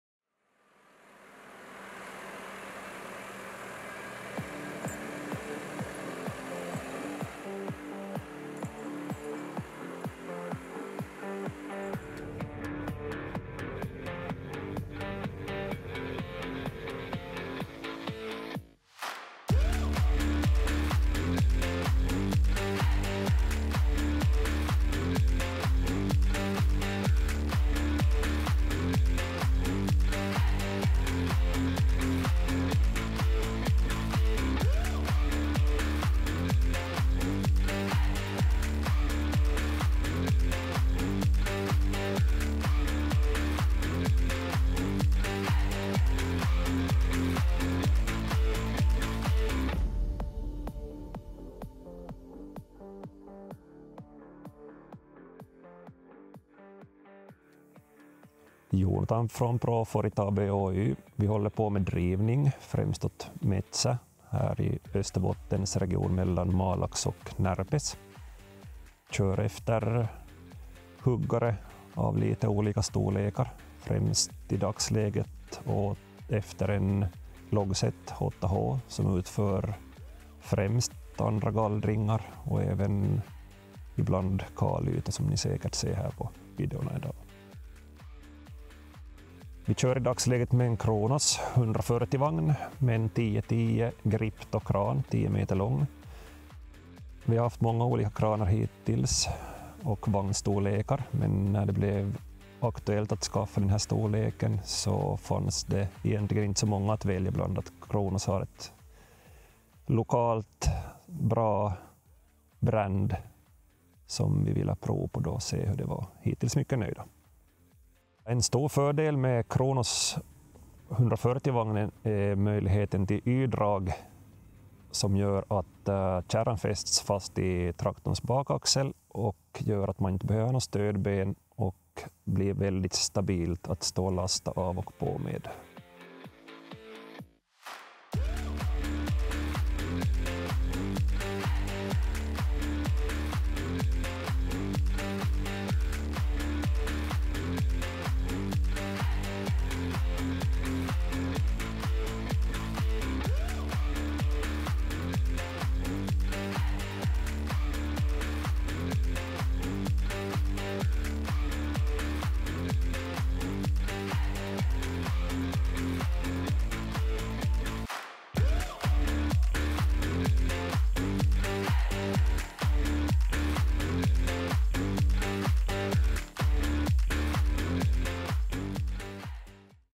Kronos 140 4WDM - ProForit Ab Oy
Kronos 140 forest trailer equipped with mechanical four-wheel drive, Y-drawbar and a Gripto 1010 crane with a reach of 10 meters.

Kronos 140 metsäperävaunu varusteltuna mekaanisella nelivedolla, Y-aisalla sekä Gripto 1010 kuormaimella jossa 10 metrin ulottuvuus.

Kronos 140 skogsvagn utrustad med mekanisk fyrhjulsdrift, Y-dragbom och en Gripto 1010 kran med 10 meters räckvidd.